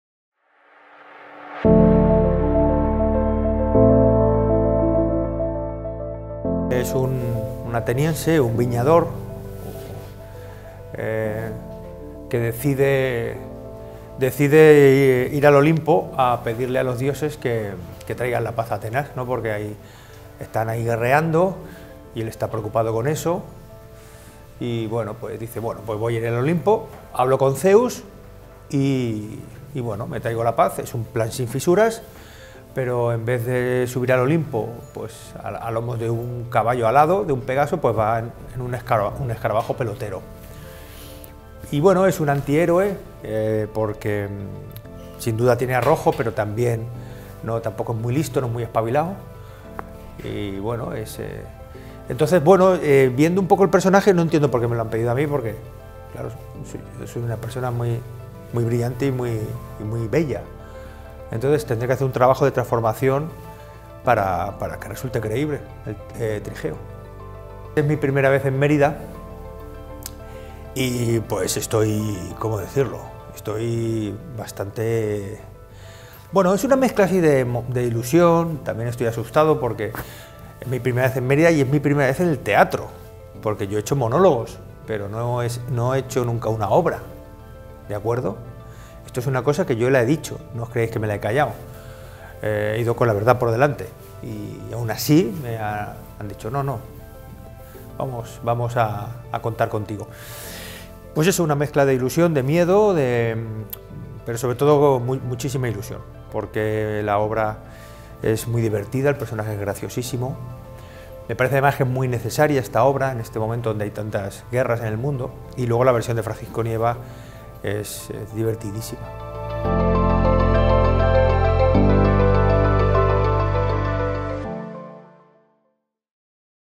𝗝𝗼𝗮𝗾𝘂í𝗻 𝗥𝗲𝘆𝗲𝘀 es Trigeo en 𝘓𝘢 𝘗𝘢𝘻 ·
Una mezcla de nervios, miedo e ilusión es lo que siente Joaquín Reyes pensando en su primera vez sobre las piedras del Teatro Romano Festival de Mérida.

Risas y diversión no faltarán en esta versión del clásico de Francisco Nieva con la interpretación de un personaje que supone toda una emocionante experiencia.

📆 Del 17 al 21 de julio en el Teatro Romano de Mérida
🎟️ Entradas en festivaldemerida.es